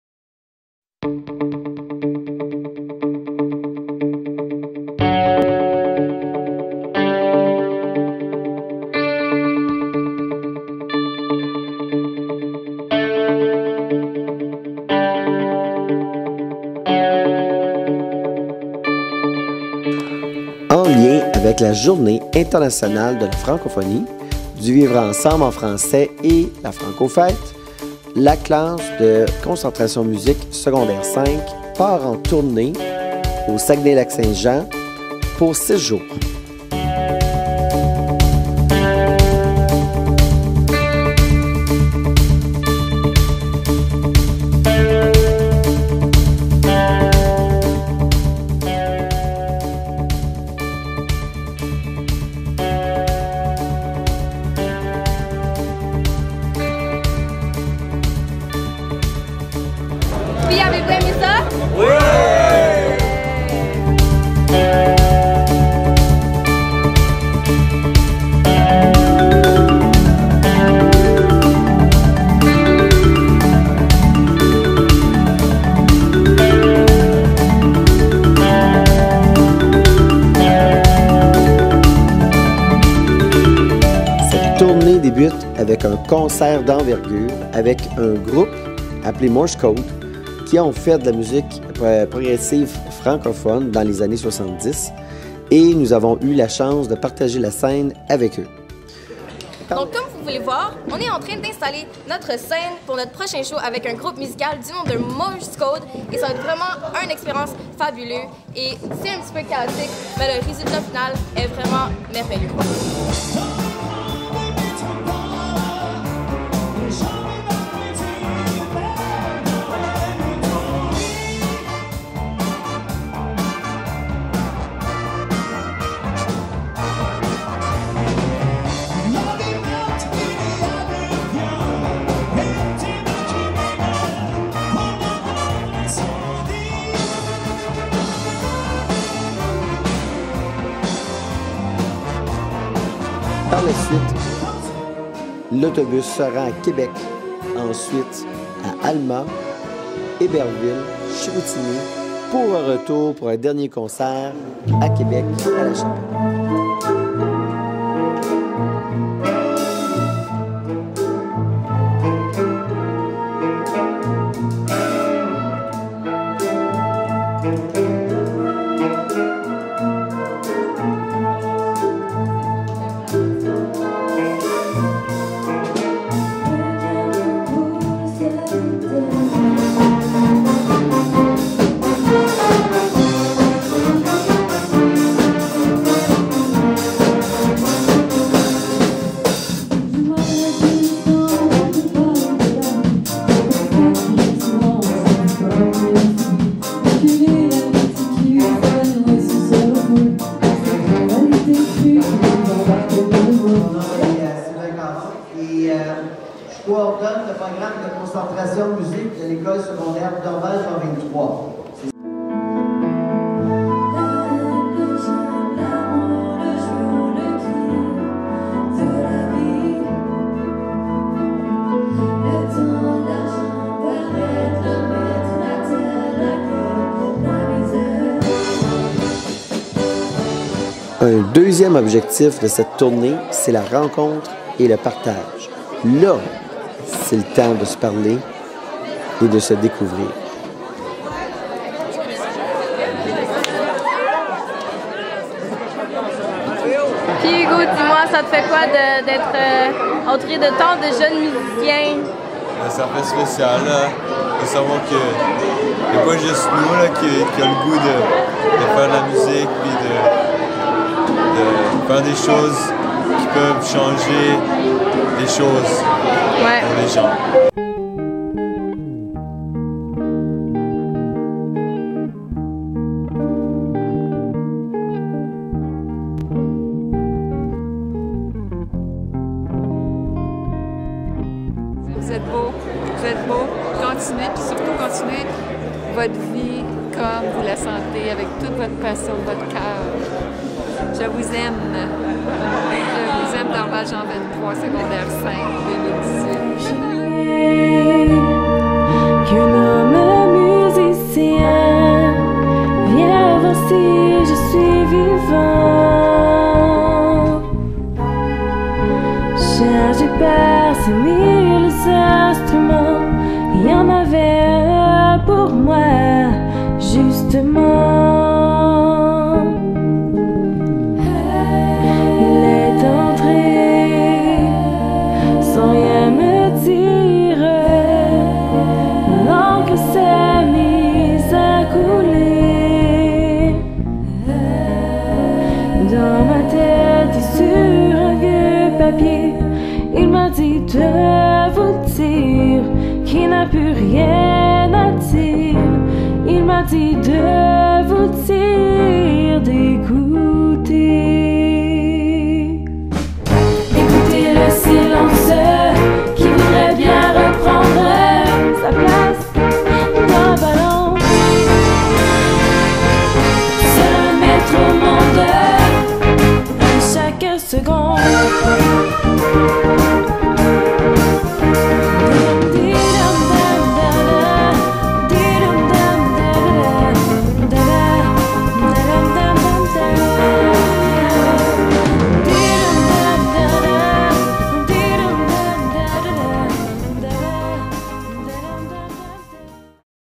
Tournée musicale Saguenay-Lac-Saint-Jean
Voici une courte vidéo cherchant à résumer l'expérience vécue par la classe de VM5 au pays des bleuets.  Merci pour votre bel accueil!